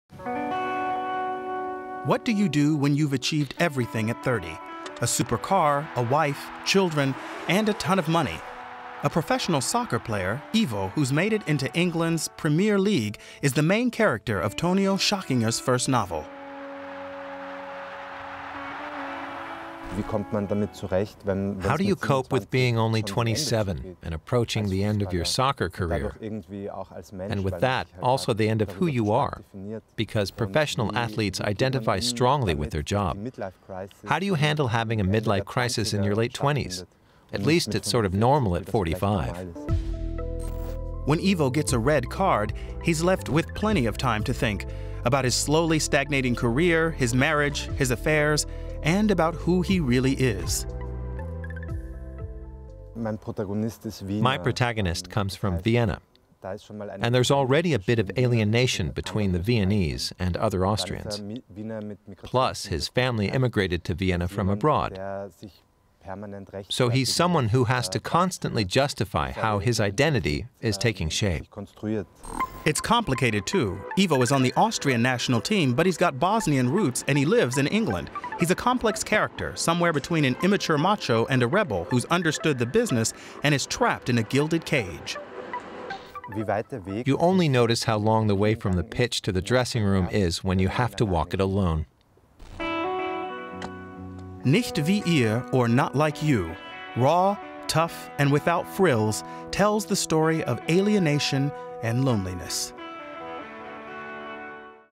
Deutscher Buchpreis 2019 | Portrait Tonio Schachinger | English
"How do you cope with being only 27, and approaching the end of your soccer career, and with that also the end of who you are?"

Portrait of Tonio Schachinger, whose novel „Not Like You“ is shortlisted for the German Book Prize 2019

Portrait auf Englisch von Tonio Schachinger, dessen Roman „Nicht wie ihr” auf der Shortlist des Deutschen Buchpreises 2019 steht

Produced by Deutsche Welle